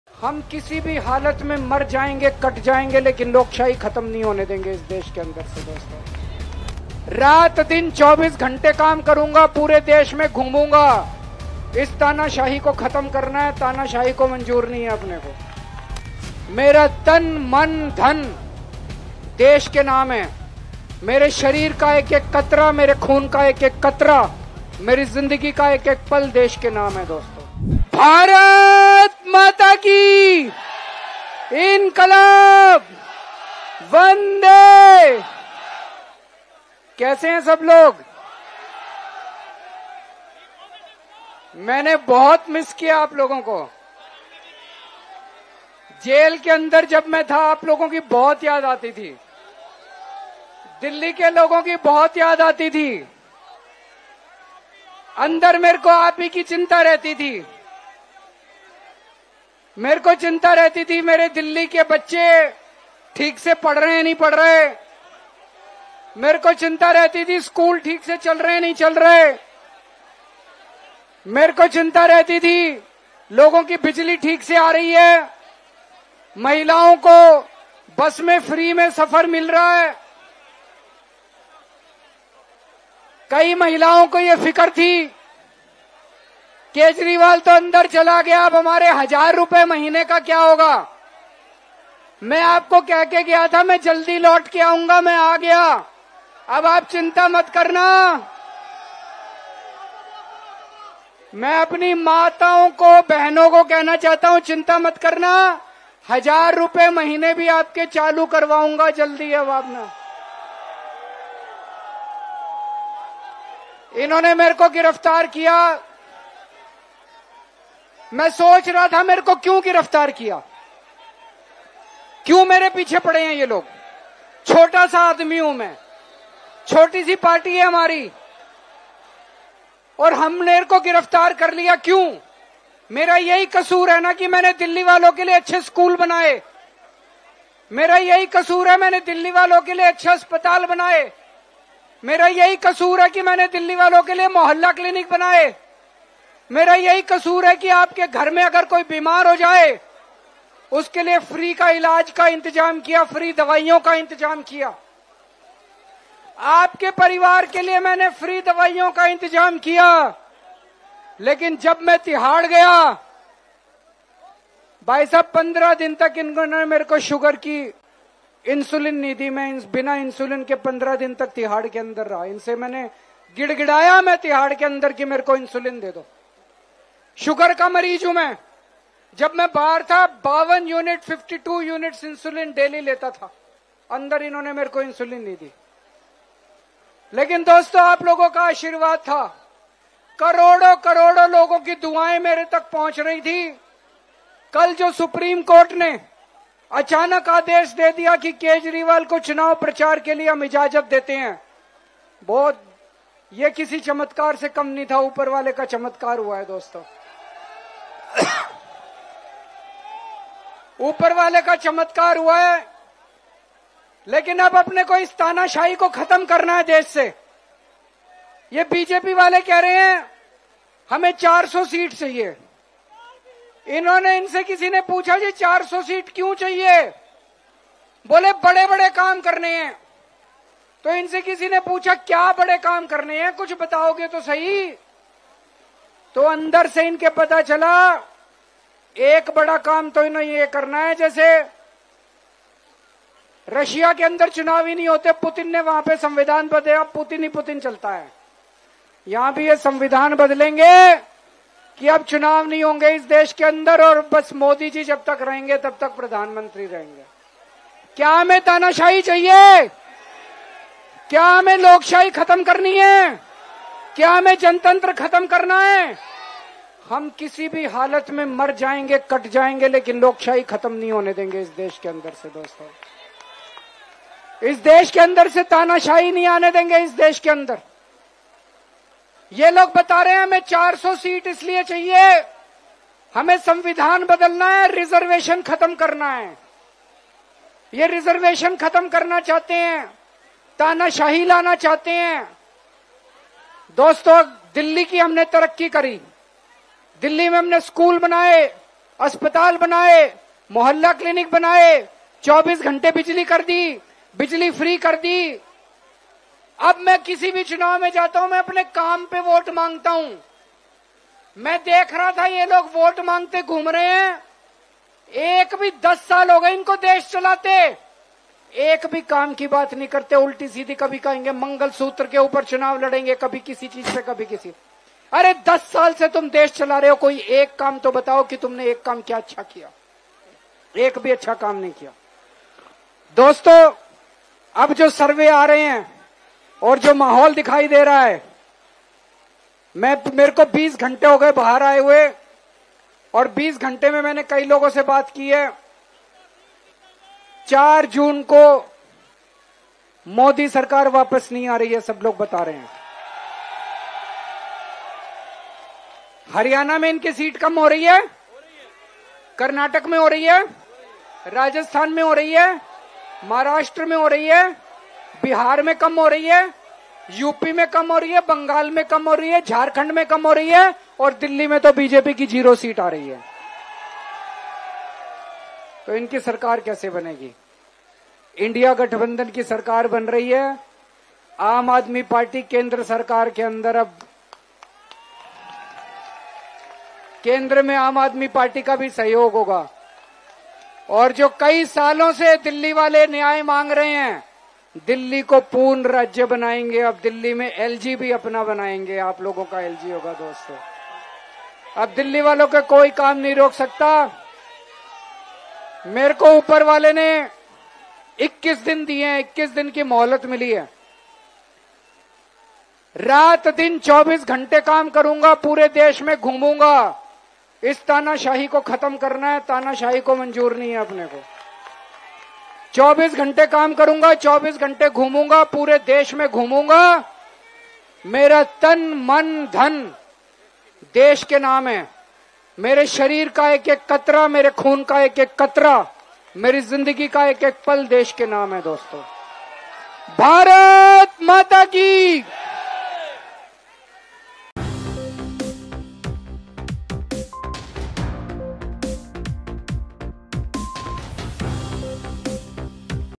माझा काय दोष होता, ज्यासाठी मला तुरुंगात टाकले - CM Arvind Kejriwal
माझा काय दोष होता, ज्यासाठी मला तुरुंगात टाकले - अरविंद केजरीवाल

'आम्ही मरू, कापले जाऊ पण लोकशाही संपू देणार नाही'